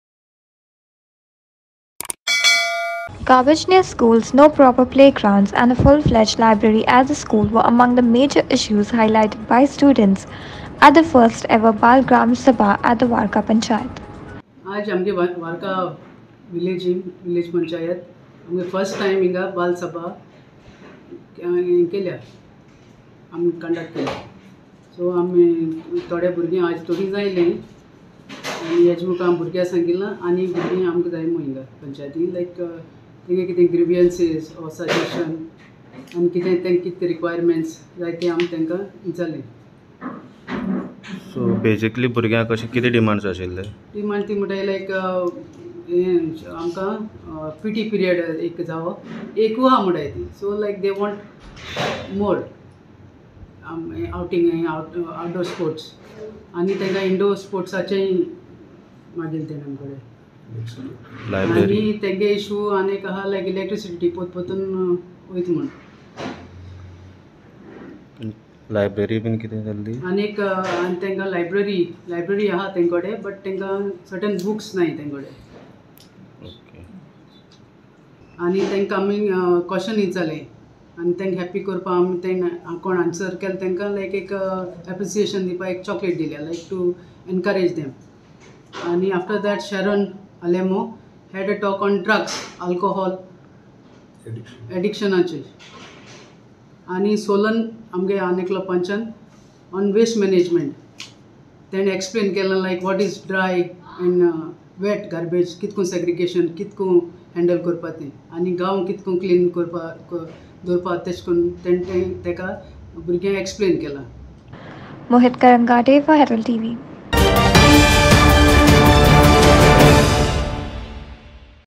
Frequent Power cuts, Better Library at schools are the main problems for students in Varca
Garbage near schools, no proper playgrounds and full fledged  library at School were among the major issues highlighted by students at the first-ever ‘Bal Gram Sabha’ at Varca Panchayat, this  Bal Gram Sabha was organised by the Varca Panchayat, wherein students from the schools within the Varca panchayat Jurdiction took part in this gram sabha and highlighted the issue they face on daily basis.